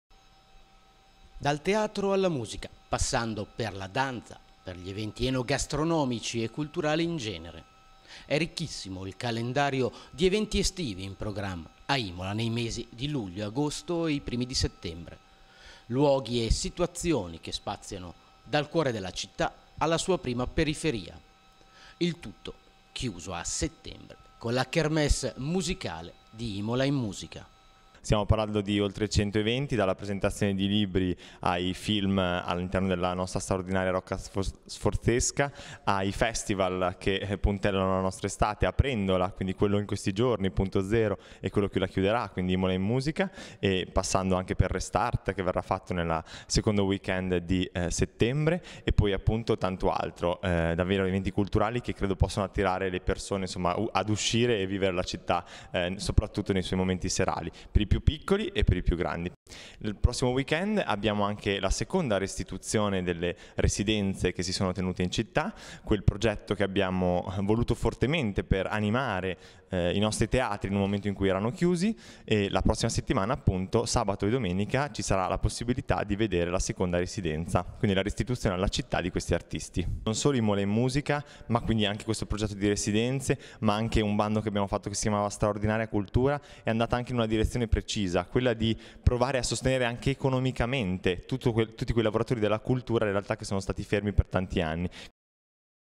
IMOLA: Un calendario ricco per le iniziative e le manifestazioni d’estate in città
Dal teatro alla musica passando per la danza, per gli eventi enogastronomici e culturali in genere. E’ ricchissimo il calendario di eventi estivi in programma a Imola nei mesi di luglio, agosto e settembre.. Luoghi e situazioni che spaziano dal cuore della città alla sua prima periferia. Il tutto chiuso a settembre con la kermesse musicale di Imola in musica